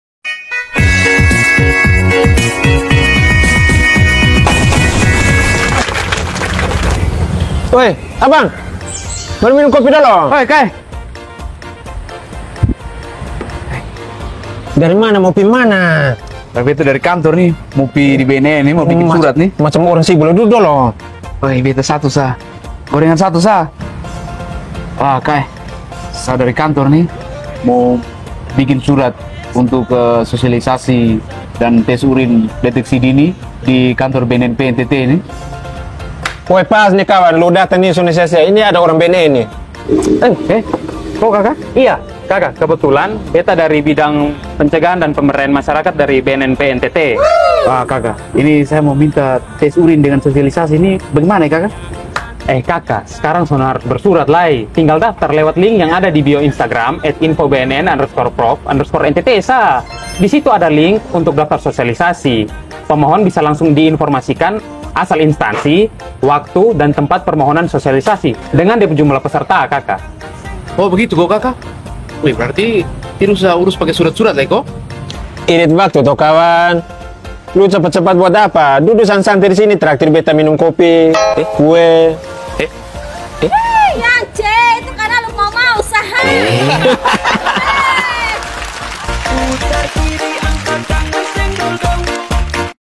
SITKOM WR. MINCE BERSINAR
Eps 8. Tahun Baru Gaya Baru,Daftar Sosialisasi deng Tes Urine Online
Karmana tu???
Simak dolo.

Tallent :
@yance_leo sebagai security
@andar_1412 sebagai bang bedhu
@thinolamak sebagai kaka yohanes
@novijanudin sebagai noja

Dericted by. Humas BNNP NTT